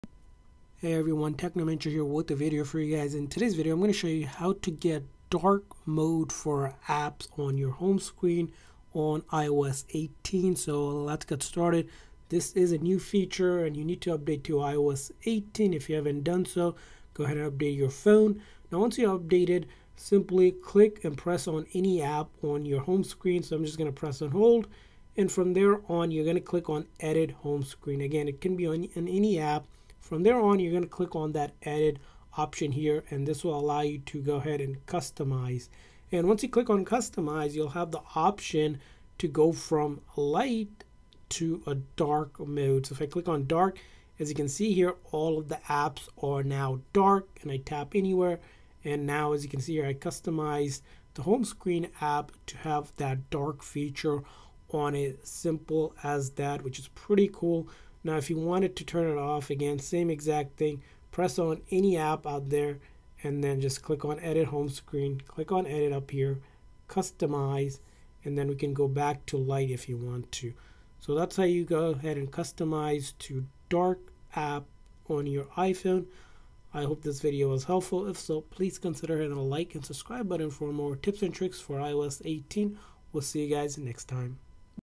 How To Get Dark Mode for Home Screen Apps iOS 18
Here's how to get dark mode for app icons on Home Screen on iPhone iOS 18!